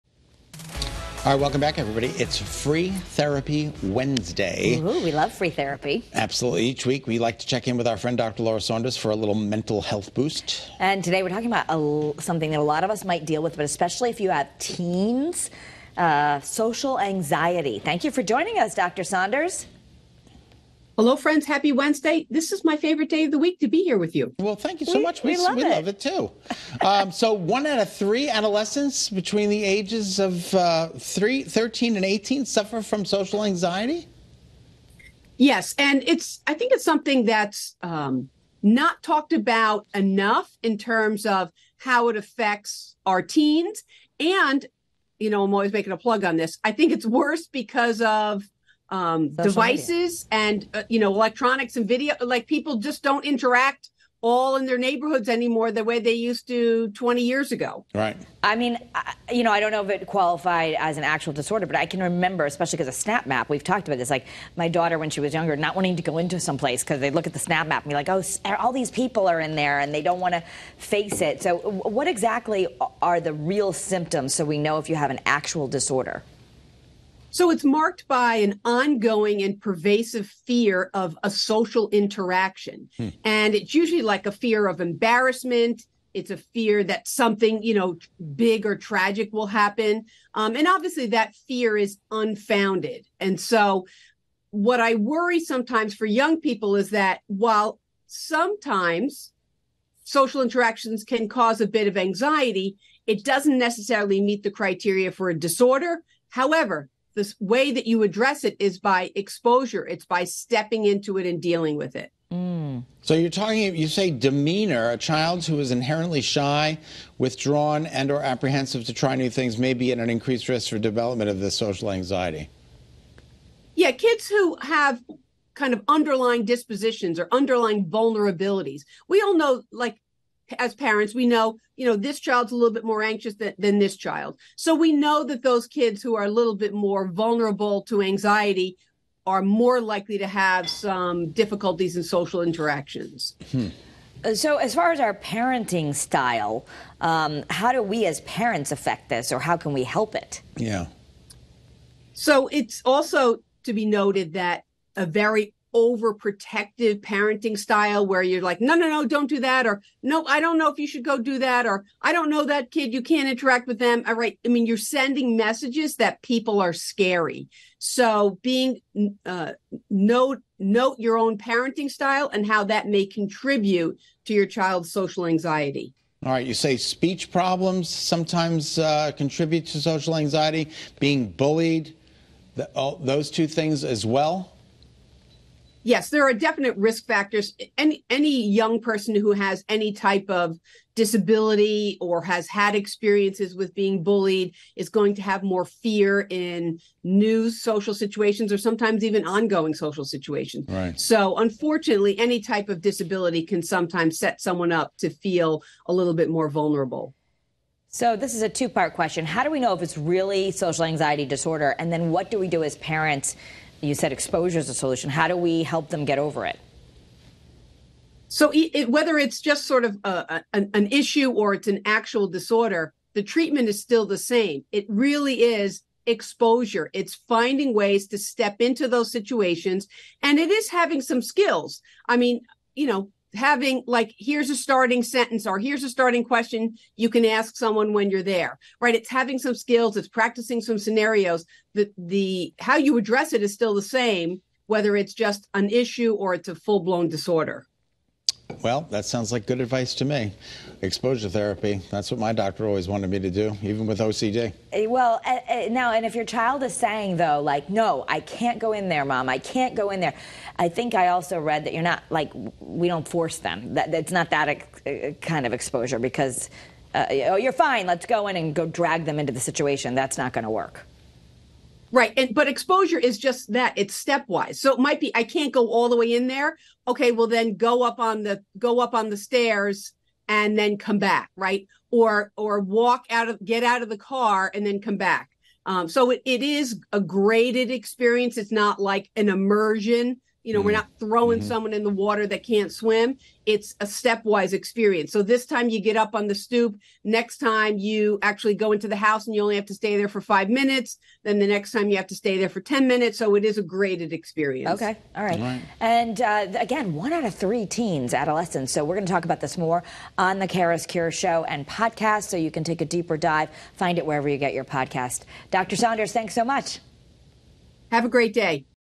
Mental Health with Dr. Saunders
Dr. Saunders is here to talk about social anxiety in teens.